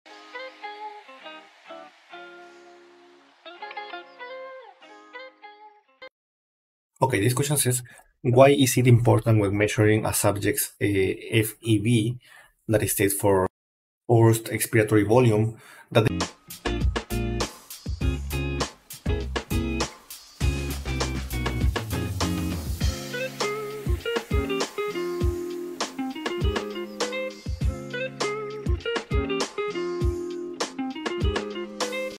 Why is it important when measuring a subject's FEV that they only perform the measurement after a m…
Why is it important when measuring a subject x27;s FEV that they only perform the measurement after a maximal inspiration? What kind of respiratory condition might they be incorrectly diagnosed with?